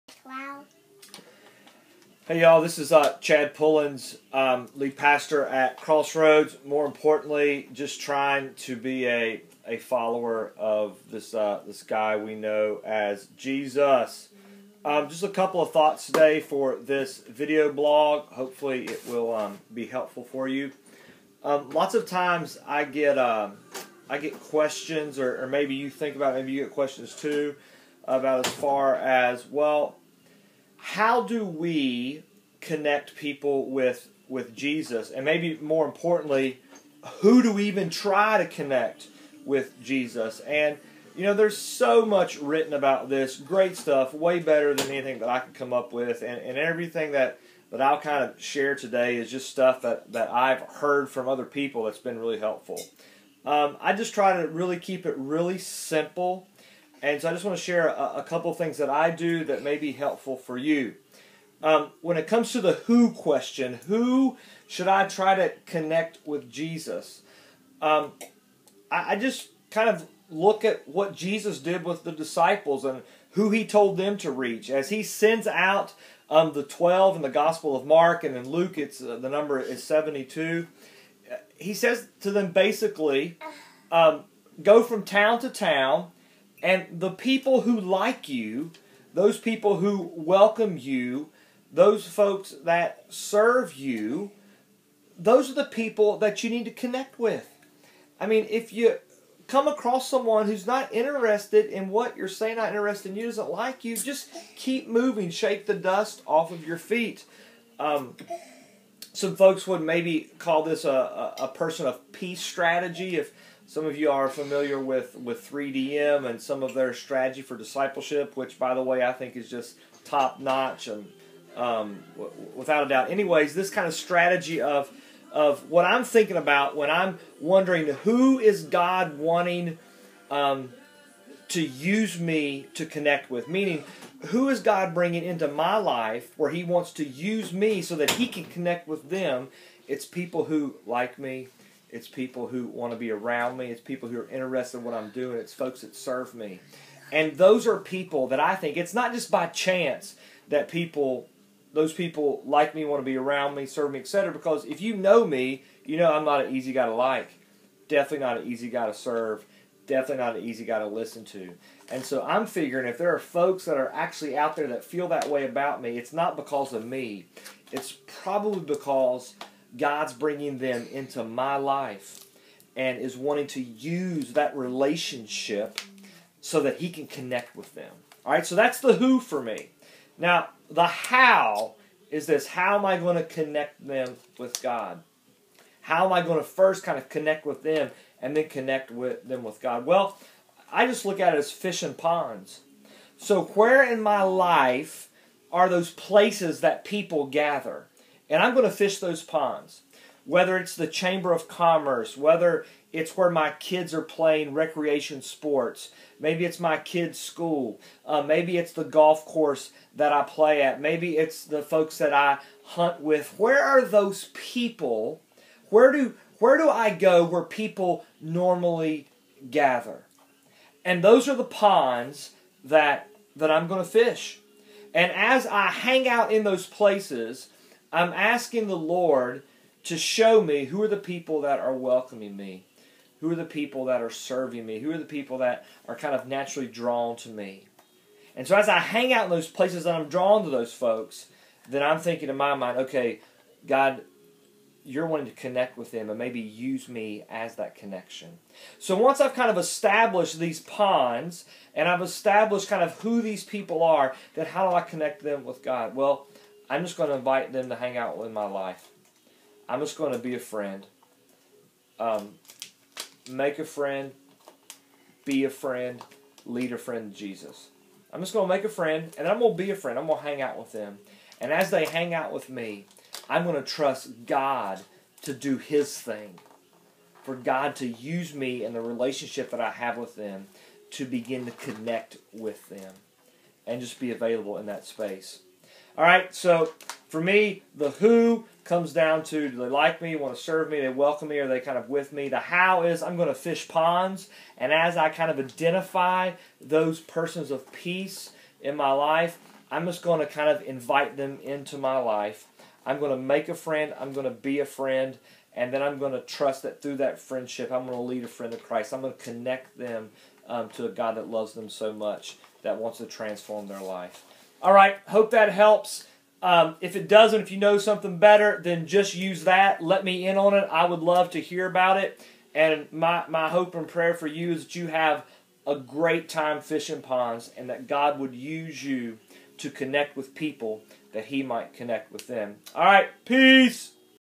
Crossroads Video Blog (fishing ponds 5-1-12)
This video blog discusses a strategy to connect with people and then connect them to Jesus through an intentional relationship